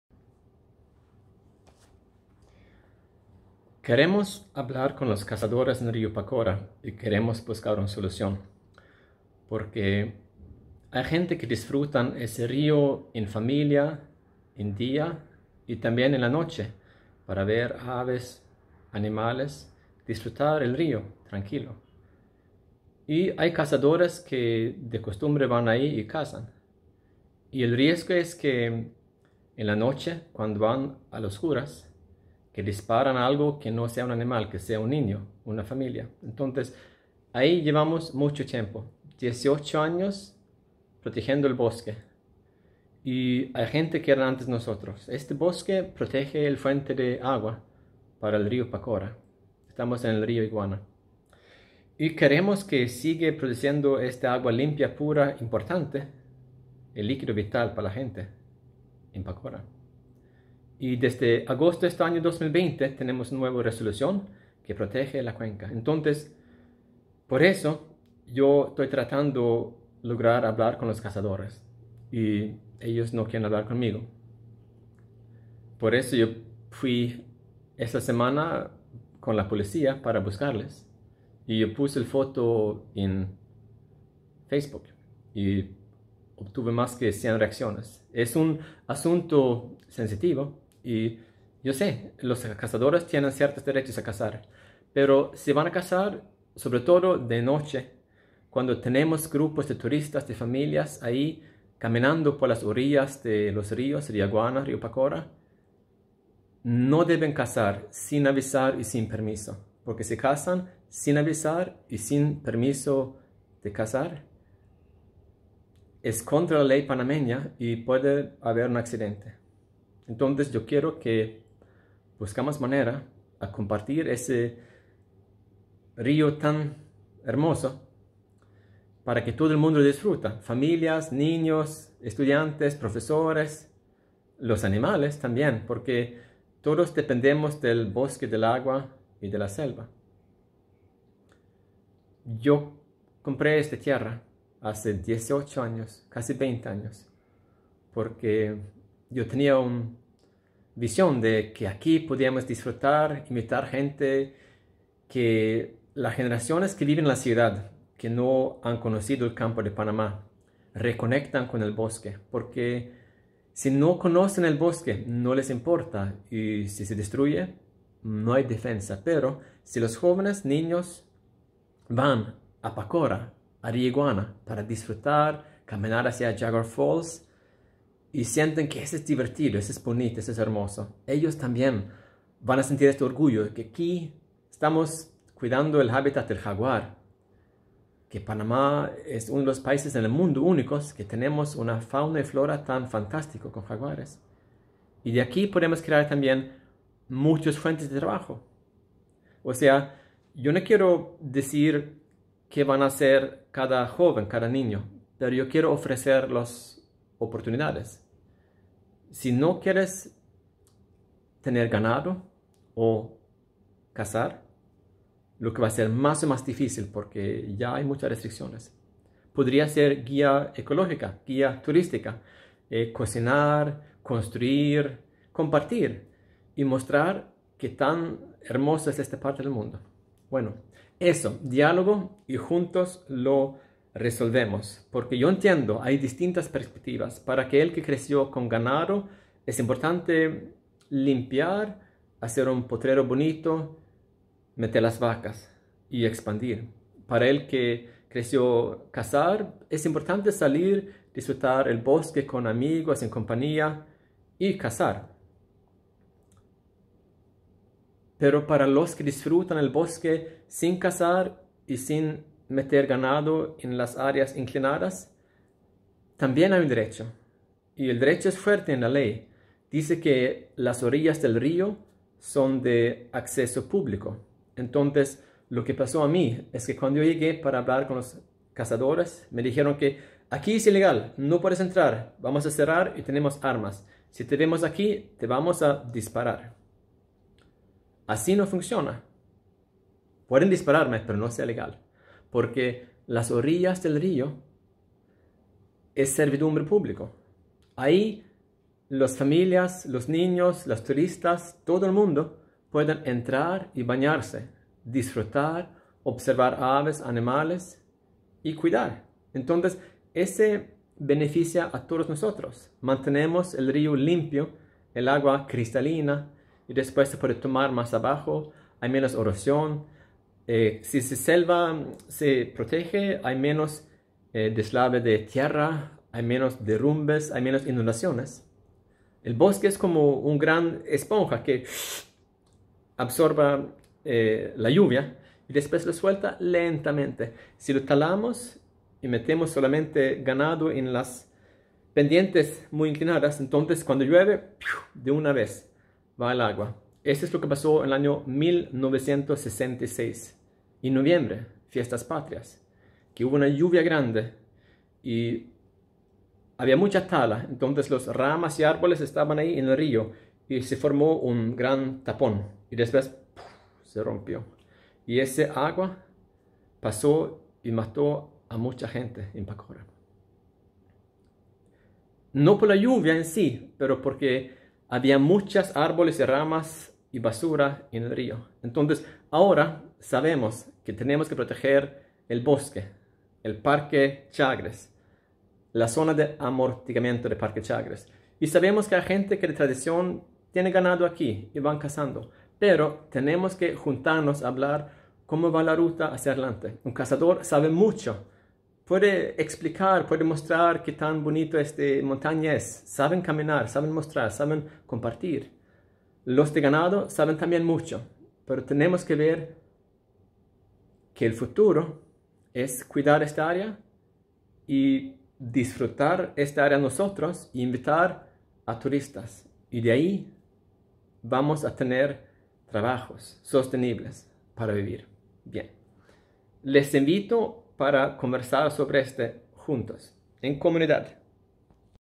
Recreacion o/y Caceria en Rio Pacora?
Como podemos juntos disfrutar y proteger el Rio Pacora de Panama?